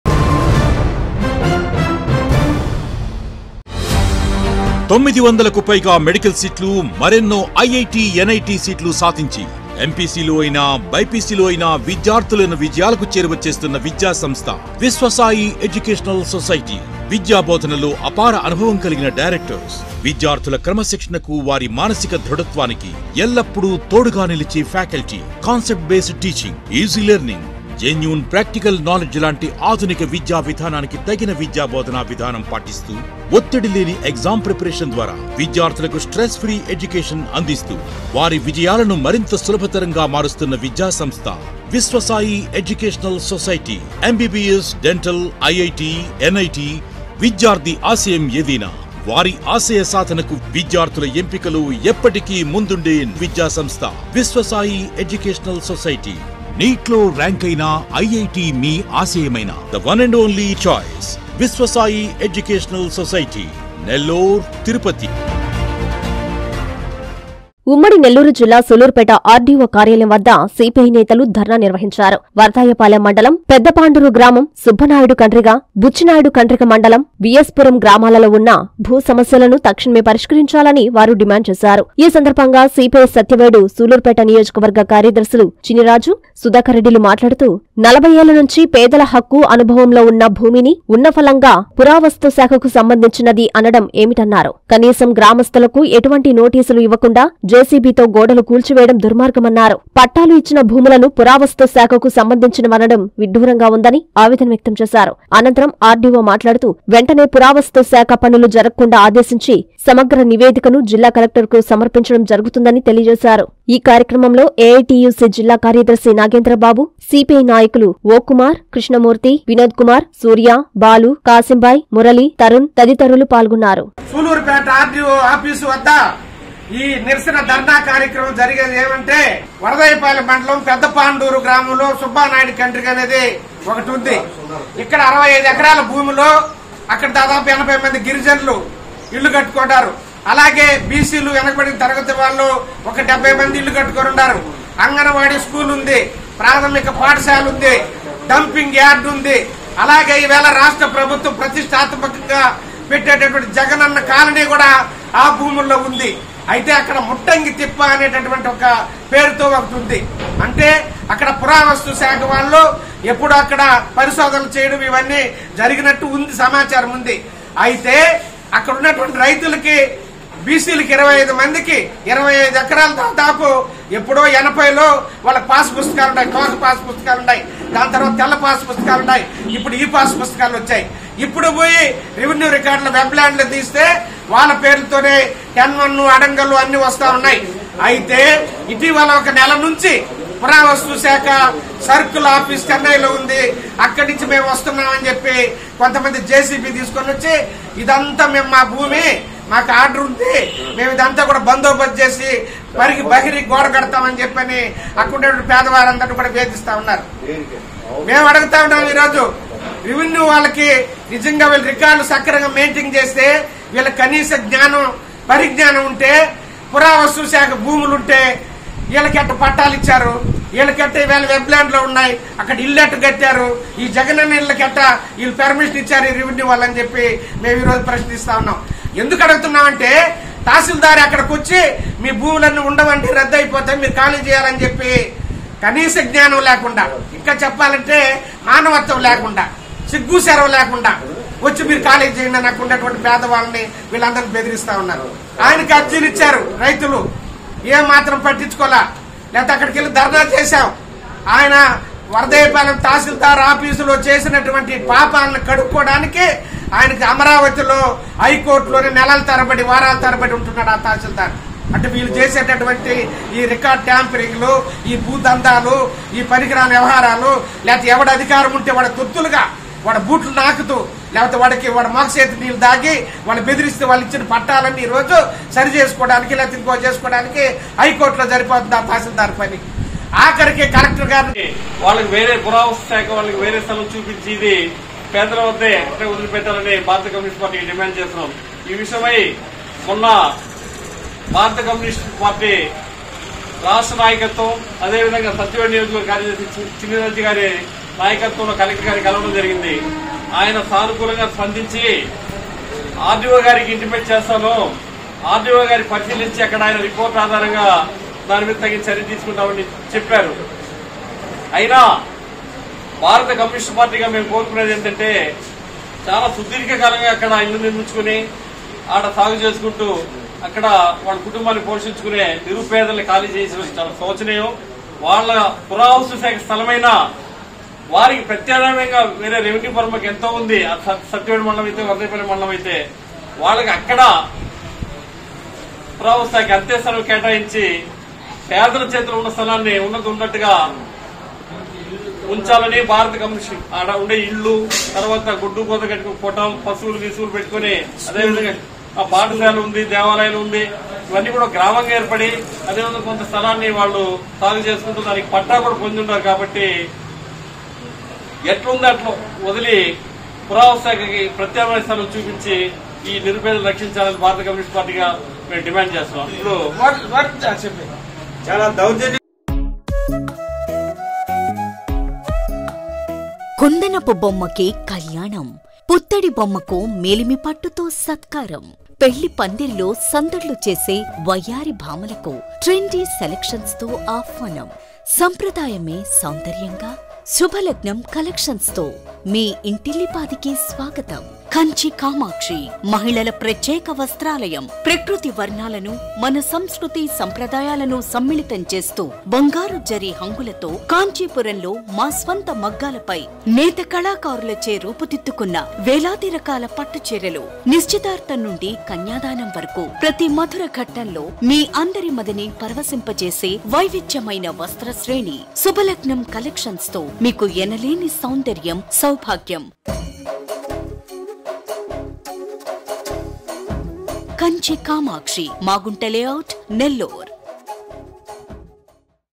భూసమస్యలపై సీపీఐ ఆందోళన
సూళ్లూరుపేట RDO ఆఫీస్ ఎదుట నిర‌స‌న‌